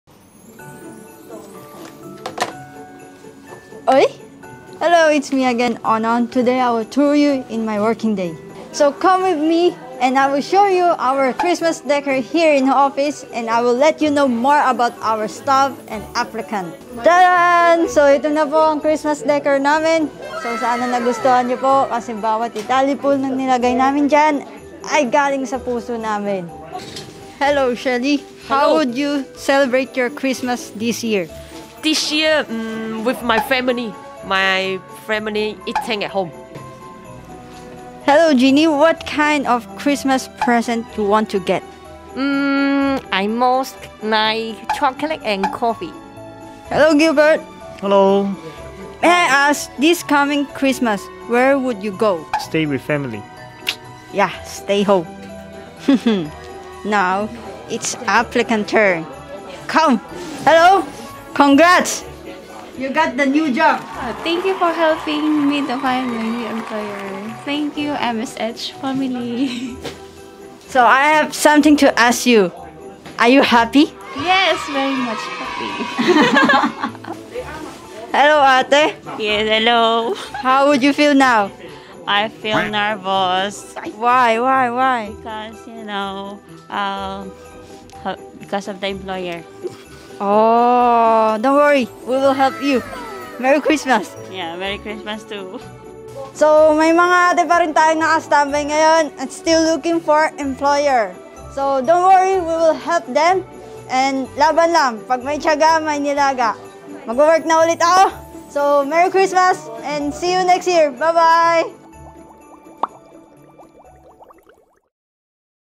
ONON VLOG : 聖誕和你Chat
Merry Christmas mga ate🎅🏻🎉! excited na din ang my sweet home na salubongin ang pag pasok ng pasko , bawat branch may ibat iba palamuti! tara samahan nyo si onon sa short tour niya at have a small talk ❤️ ano kaya ang mga wish nila ngayon pasko🥰?